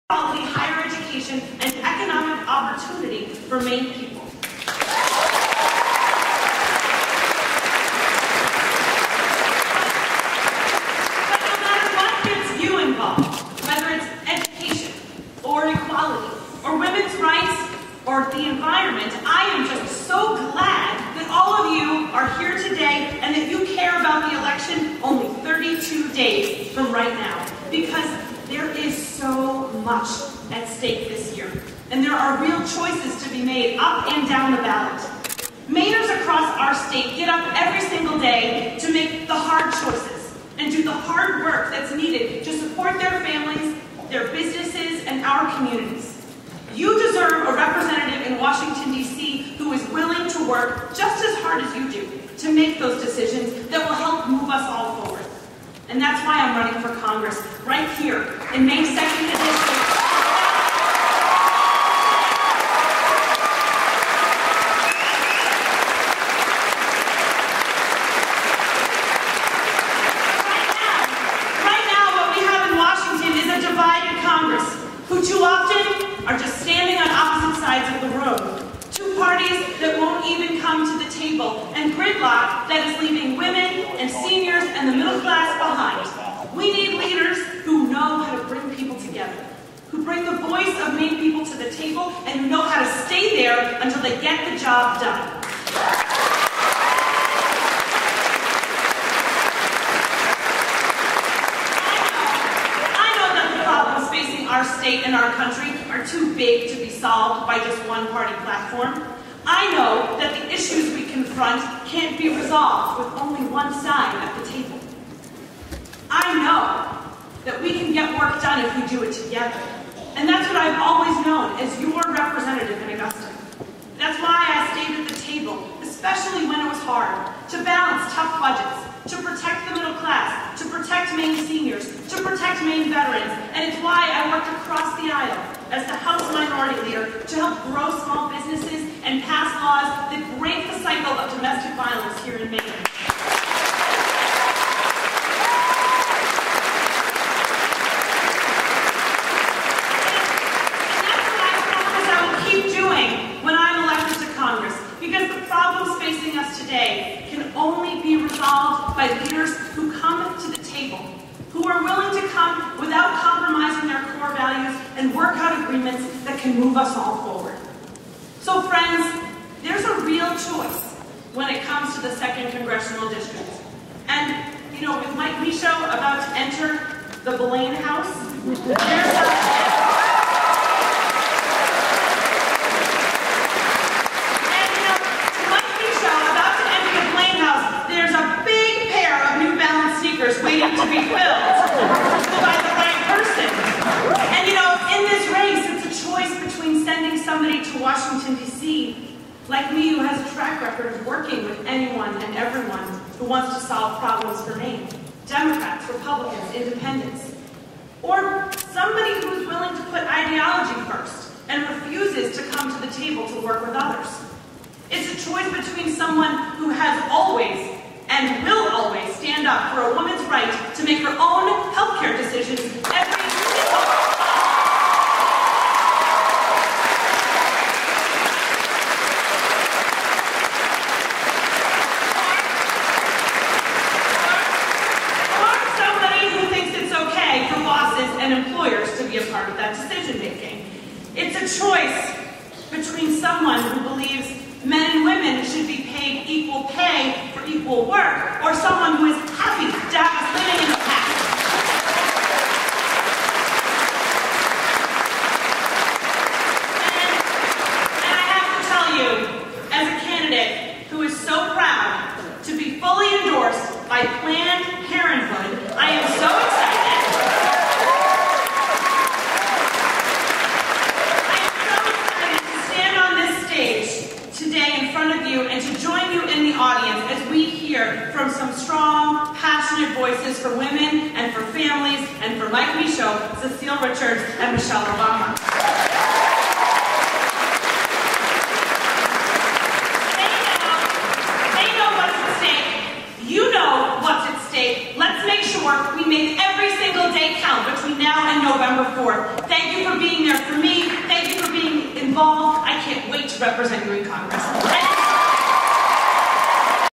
Emily Cain for Congress ME-CD2 Addresses Crowd at UMaine Orono
Speech given 10/3/14 before a packed house at FLOTUS/ Mike Michaud rally